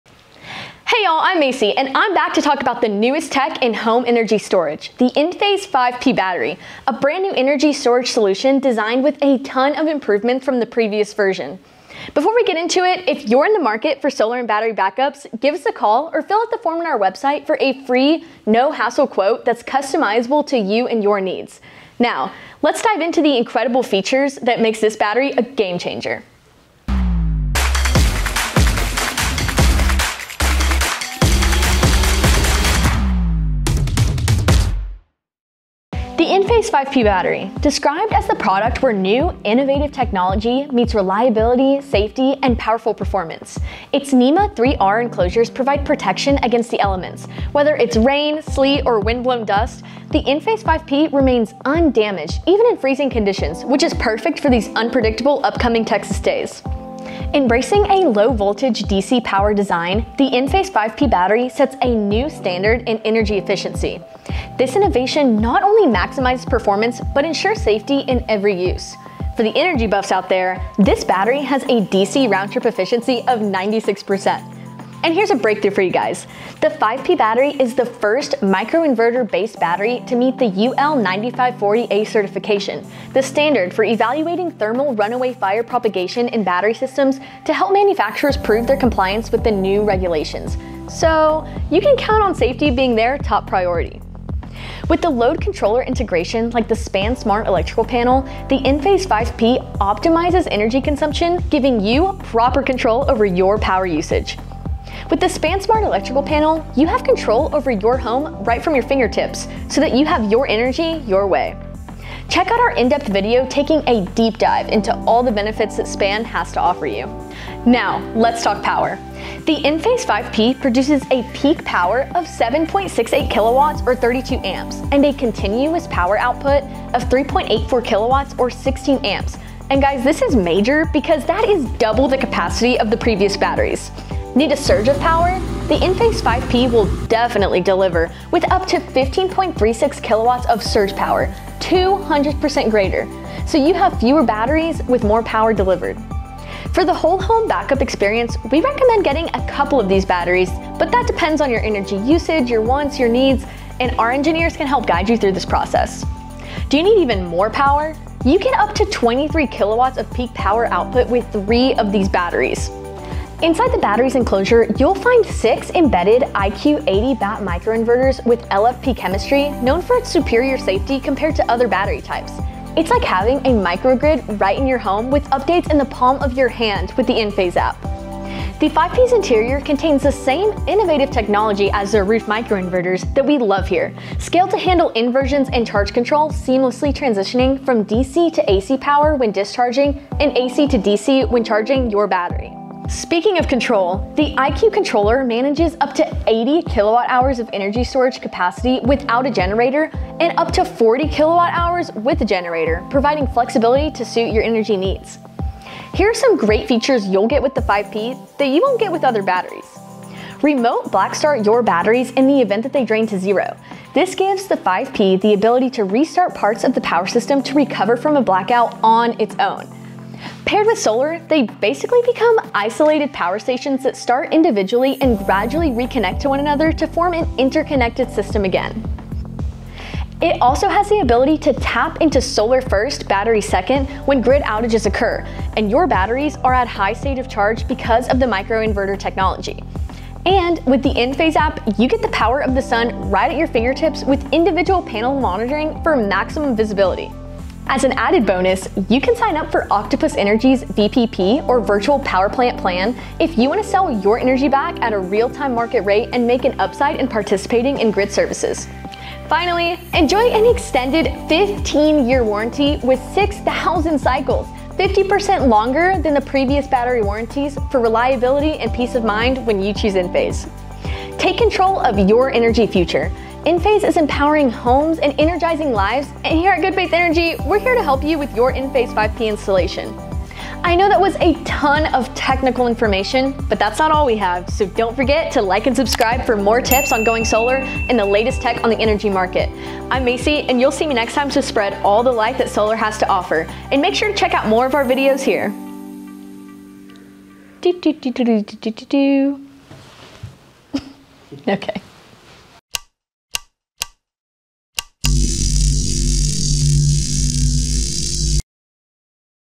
Enphase IQ Battery 5P | Technical Breakdown
Unveiling the Enphase IQ Battery 5P! 🌞⚡ Explore the cutting-edge features revolutionizing home energy storage. Designed for reliability and safety, this new solution sets new standards in performance. If you're considering solar and battery backups, reach out for a hassle-free quote tailored to your needs.

Key Features:
-NEMA 3R enclosures for all-weather protection.
-Low voltage DC power design with 96% round trip efficiency.
-UL9540A certified micro inverter-based battery prioritizing safety.
-Integration with SPAN Smart Electrical Panel for optimized energy consumption and control.

Power Highlights:
-Peak power of 7.68 kW and continuous output of 3.84 kW.
-Impressive surge power delivery of up to 15.36 kW (200% greater).
-Achieve up to 23 kW peak power output with 3 batteries.

Internal Tech:
-SIX embedded IQ 80-BAT microinverters with LFP chemistry for superior safety.
-IQ controller managing up to 80 kWh energy storage capacity without a generator.

Unique Features:
-Remote Black Start for autonomous recovery from blackout scenarios.
-Solar-first, battery-second approach during grid outages.
-Enphase App for individual panel monitoring.

Bonus Opportunities:
-Join Octopus Energy's VPP for real-time market rate returns.
-Extended 15-year warranty with 6,000 cycles, ensuring reliability and peace of mind.

Ready to take control of your energy future? Enphase empowers homes and energizes lives. Contact Good Faith Energy for expert assistance in your Enphase 5P installation. 🌟🔋

To learn more about the Enphase IQ Battery 5P, fill out the form on our